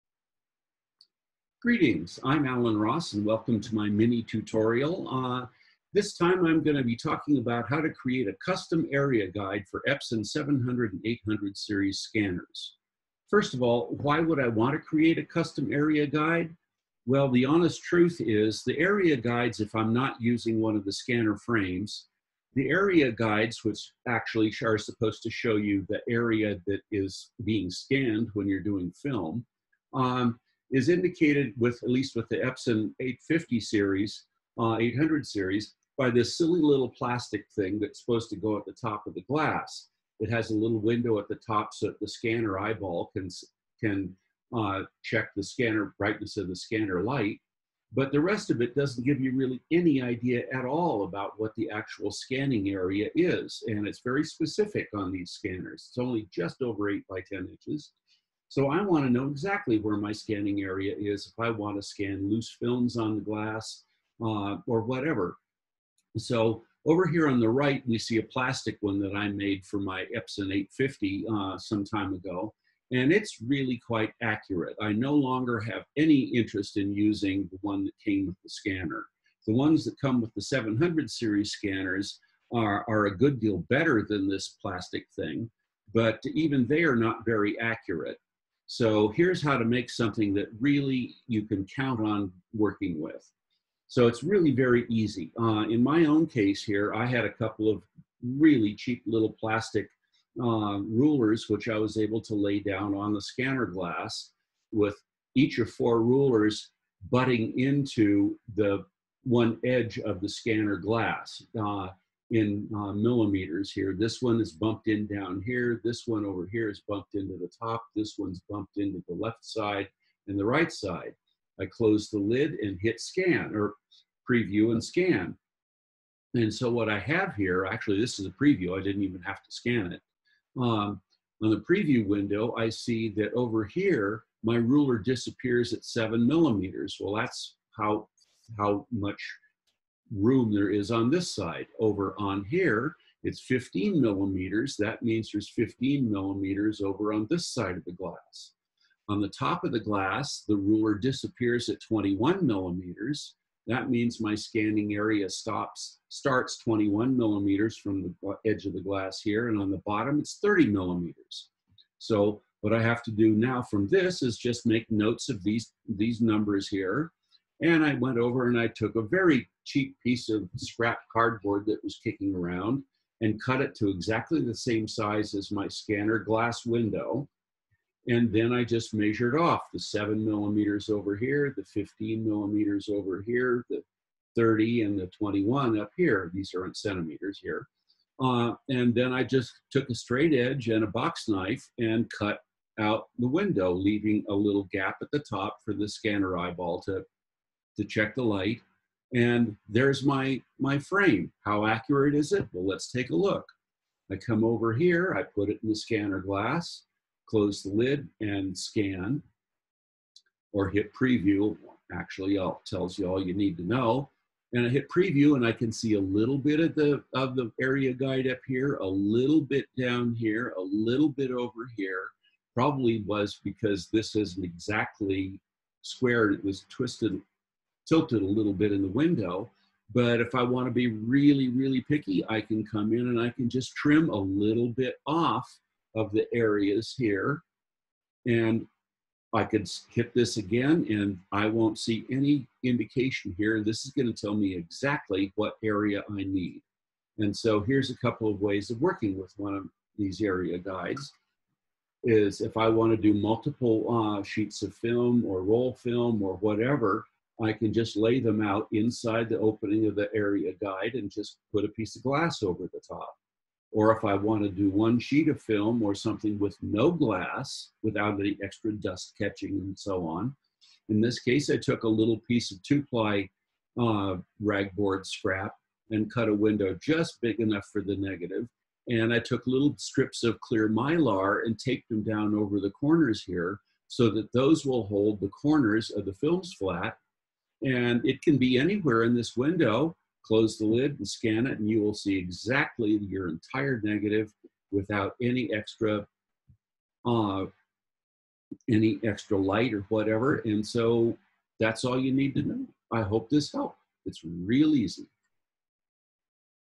Creating a Custom Film Scanning Area Guide for Epson 700 and 800 series Scanners
The actual film-scanning area on Epson 700 and 800 series scanners is smaller than the scanning glass and is not very well defined.  Here is a quick and easy way of making a custom Area Guide so you know exactly where to place your film for scanning!